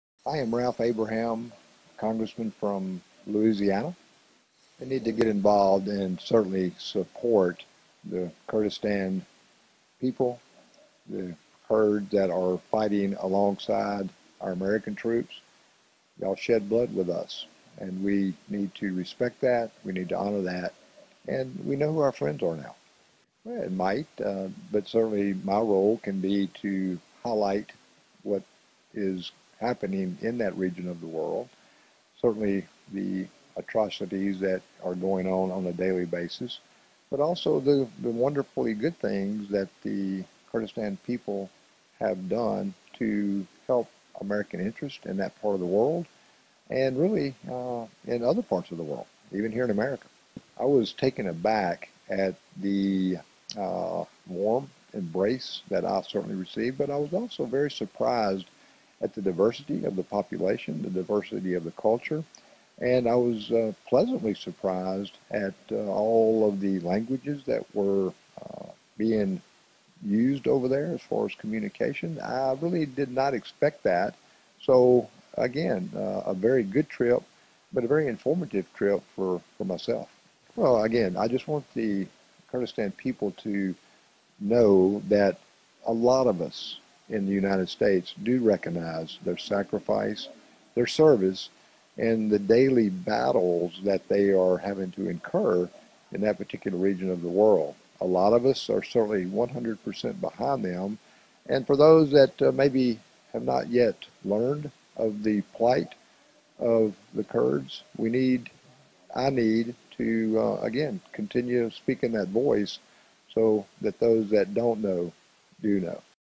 Rep. Ralph Abraham: US Needs to Support People of Kurdistan
Rep. Abraham spoke with K24 just before the showing of Bernard-Henri Levi's wonderful film, "Peshmerga", at the United States Capitol Visitor Center